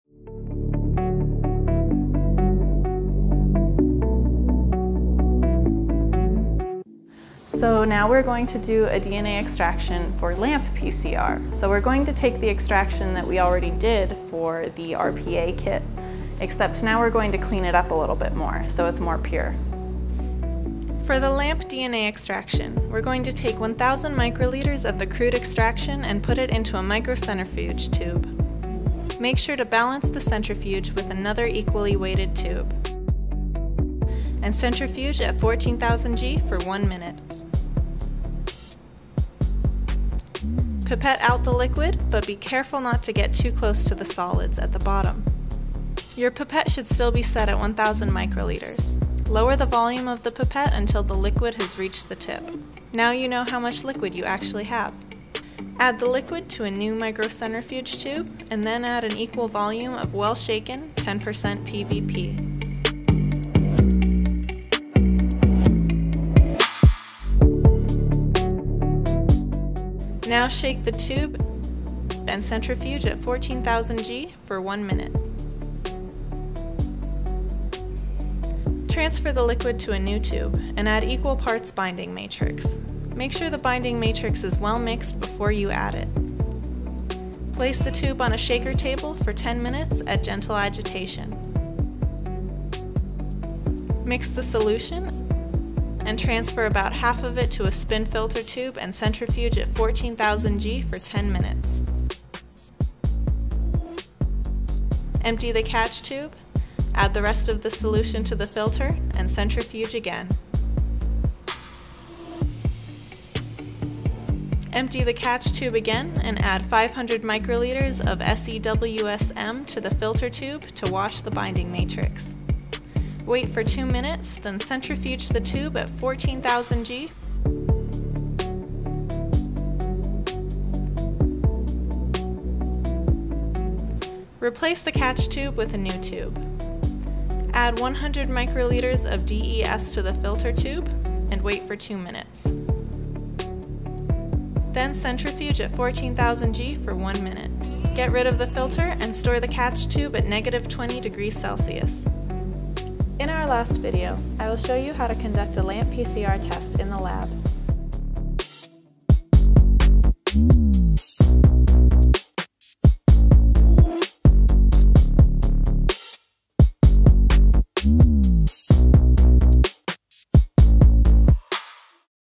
Purified DNA Extraction for Fusarium Wilt of Lettuce
Amy Pierce, a University of Arizona Biosystems Engineering student, shows how to further purify the crude sample that was created in the first video. The sample can then be used for other types of analysis in the lab.